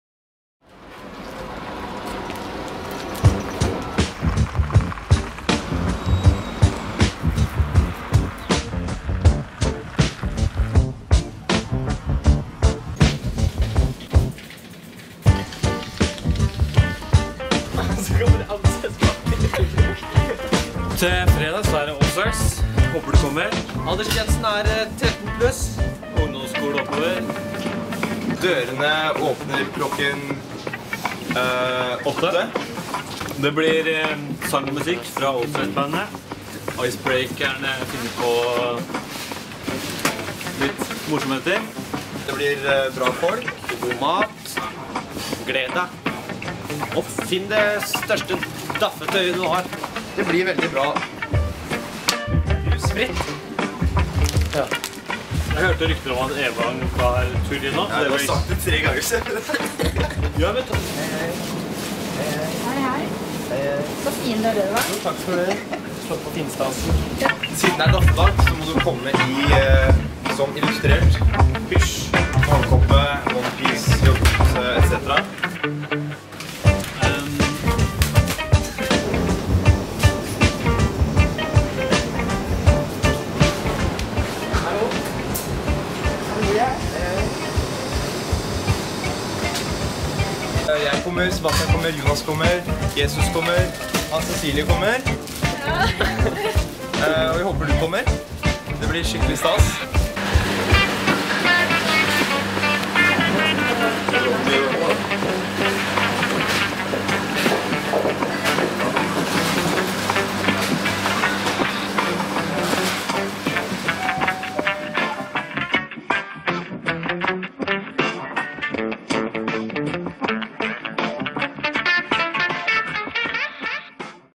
Allstars daffe-dag
Denne fredagen er det daffe-dag på Allstars. Kom som du er - i pysj, joggebukse eller onepiece. Her skal det daffes!

Vegar deler noen sanne og gode ord, det blir musikk, icebreaker og vi gleder oss til å se deg der.

Det blir muligheter for å kjøpe mat. Aldersgrense 13 år. Rusfritt.